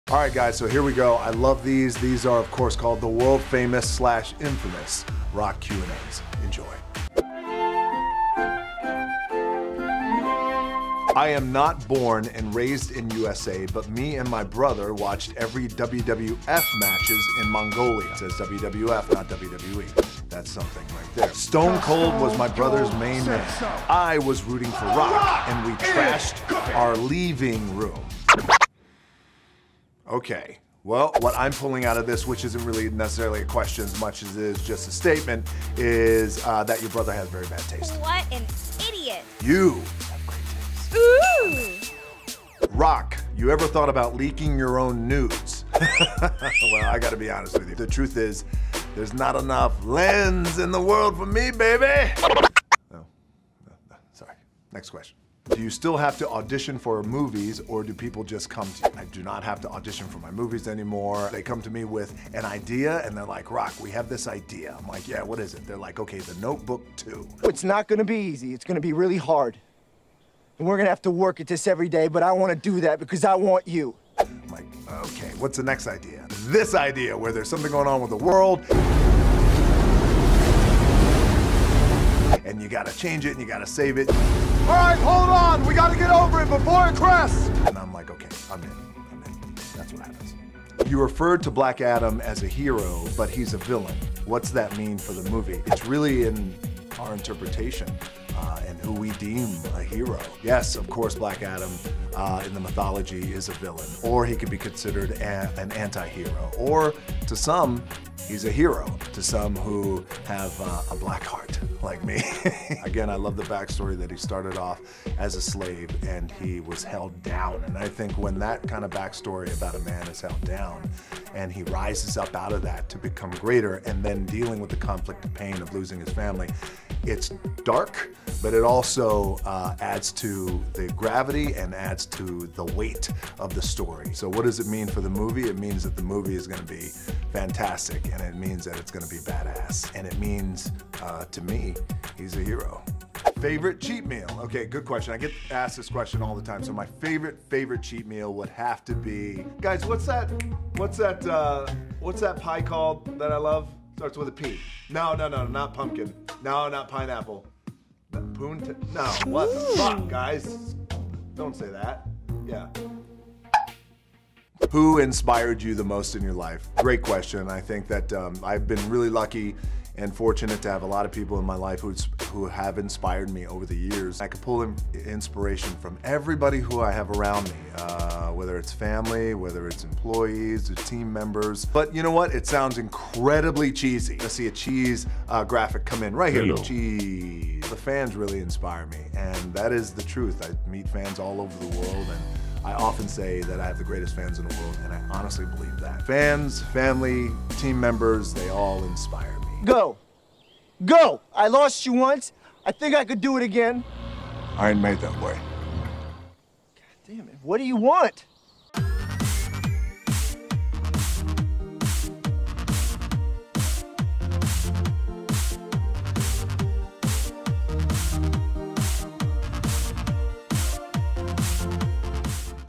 The Rock's Black Adam: A Hero? - Seven Bucks February Q&A
You have questions and Dwayne "The Rock" Johnson has answers in the February Q&A.

Executive Producers - Dwayne Johnson, Dany Garcia, Erin Lardy
Executive Producers - Scott Brown, Mo Darwiche, Dan Weinstein

CREW
Director: Scott Brown
Producers: Mo Darwiche & Erin Lardy
Director of Photography: Dylan Sachse
Editor: Ryan Moody
Production Manager: Troy Guthrie
Sound Mixer: Thomas Snodgrass
Grip - Dana Fytelson
Key Art: Natalie Roman